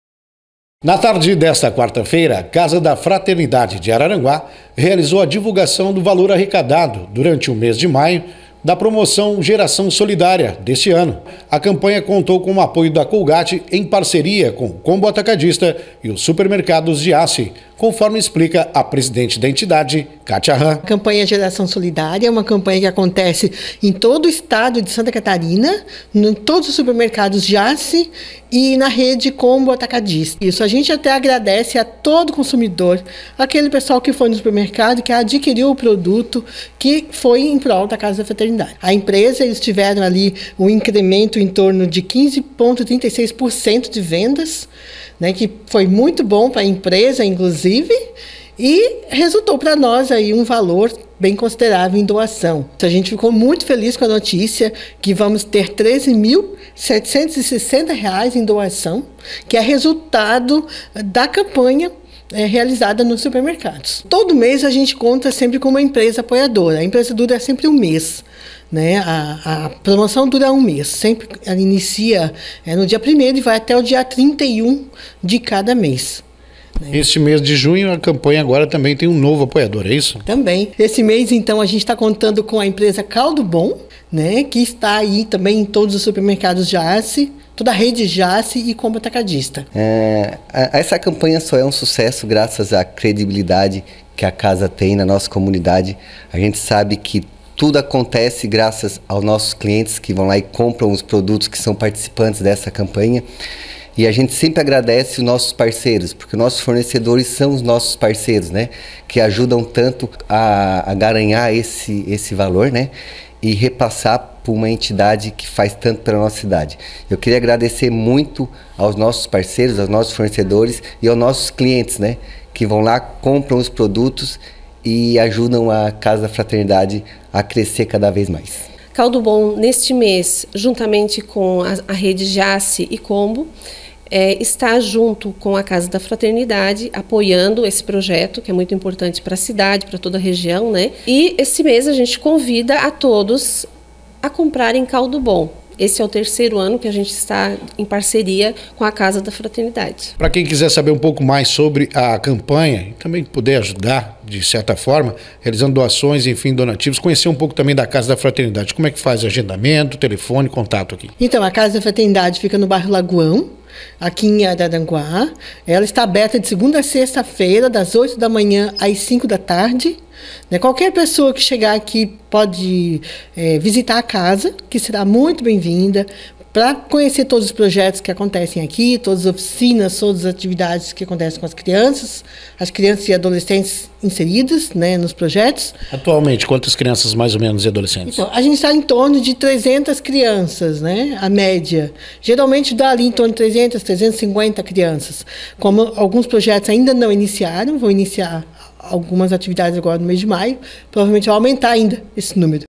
01/06/22 CASA DA FRATERNIDADE DIVULGOU RESULTADO DA PROMOÇÃO GERAÇÃO SOLIDÁRIA DO MÊS DE MAIO
Reportagem: Juliano Mendonça
Data: 01/06/22